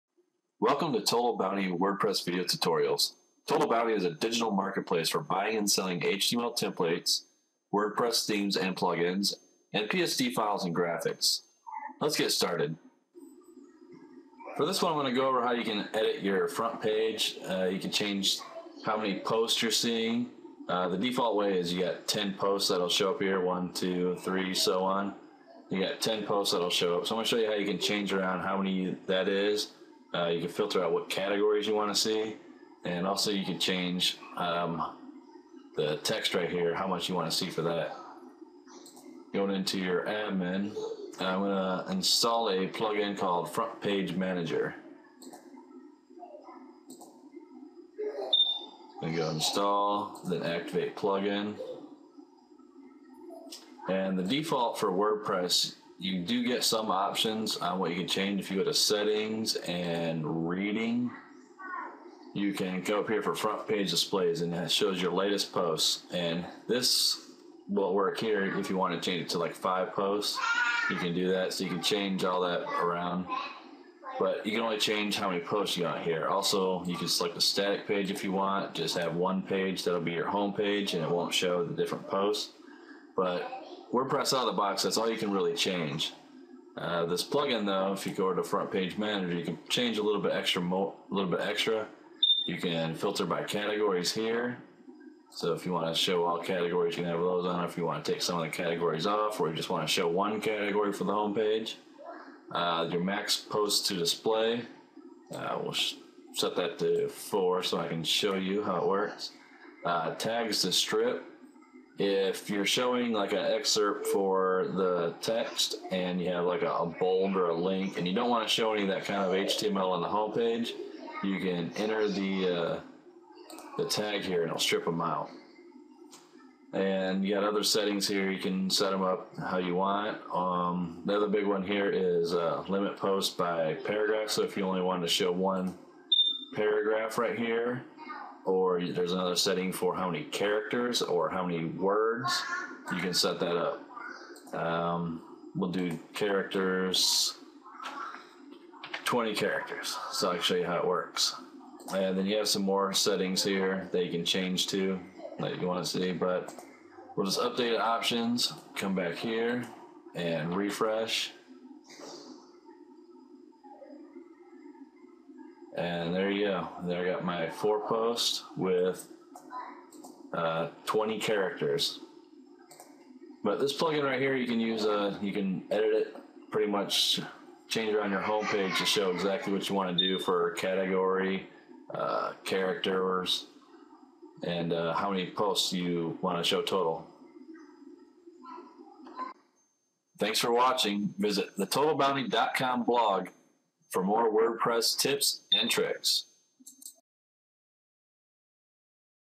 WordPress Tutorial - How to limit Posts per Page
WordPress Tutorial on how to limit the number of posts displayed per page.  Ever wanted to have a single post, 5 posts, 20 posts, or limit the posts displayed by a category?  We show you how!